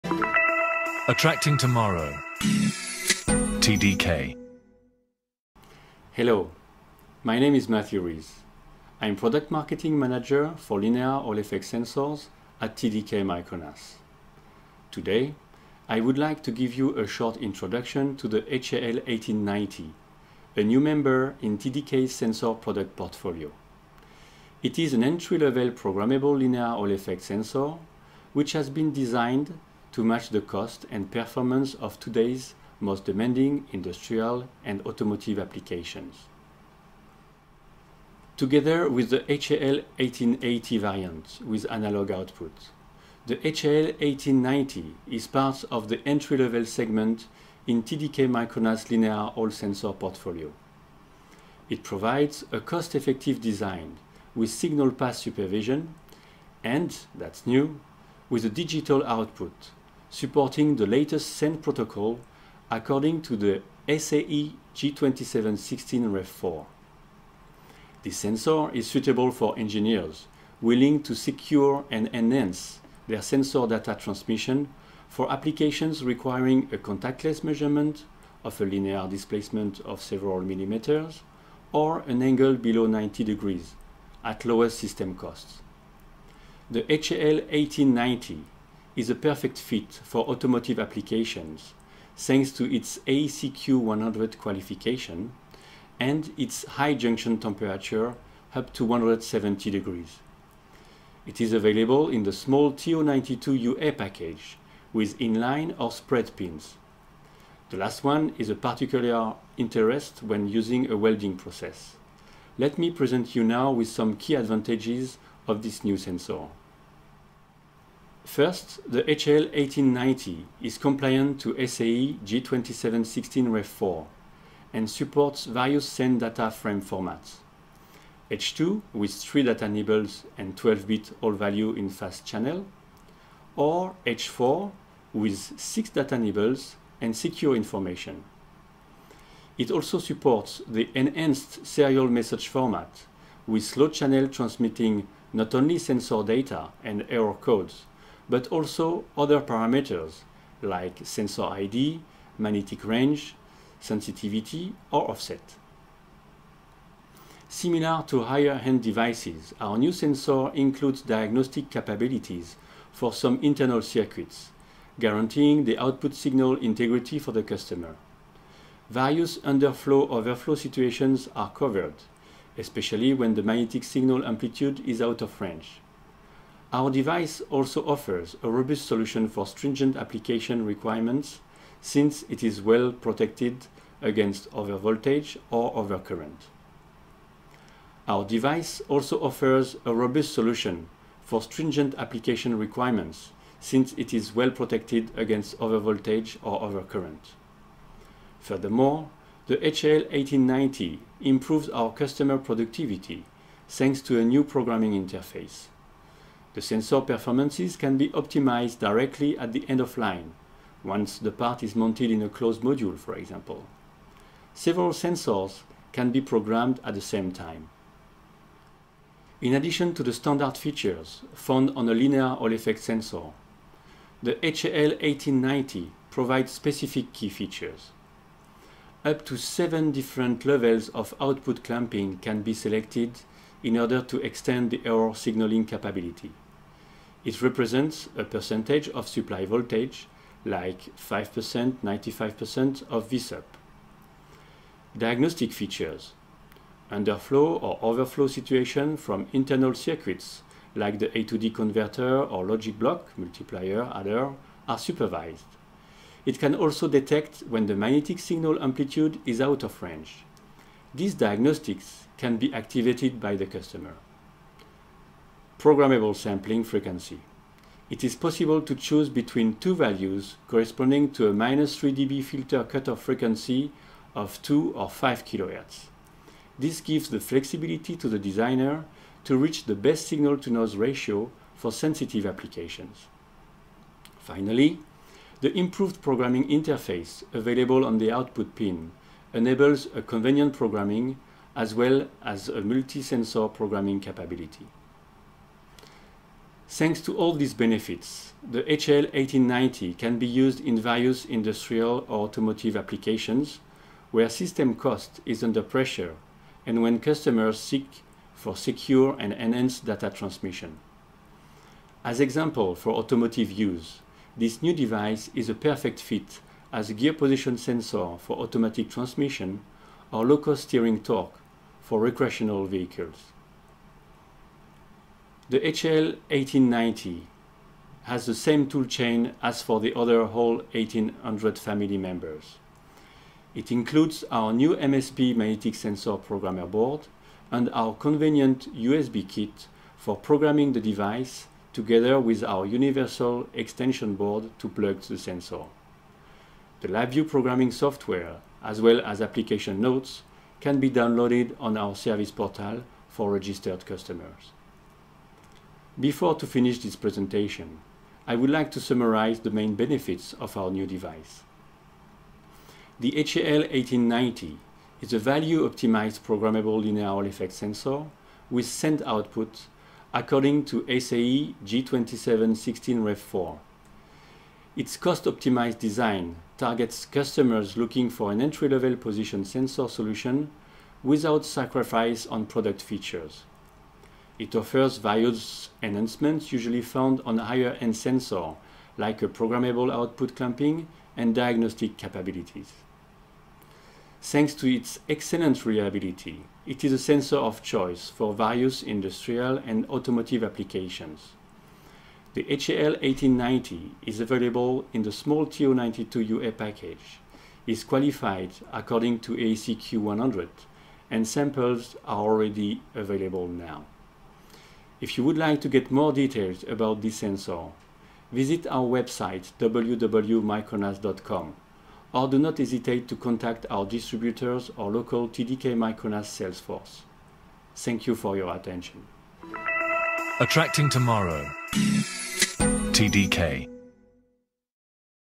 HAL1890 - Cost-Effective Linear Hall Sensor with SENT Output | TDK-Micronas
HAL 1890: Entry-level Programmable linear Hall-effect sensor with SENT output. It is a small, robust and cost effective magnetic sensor with output signal supervision capabilities. The sensor supports various SENT data frame formats (H.2 and H.4) as well as error signal transmission on Fast and Slow channels.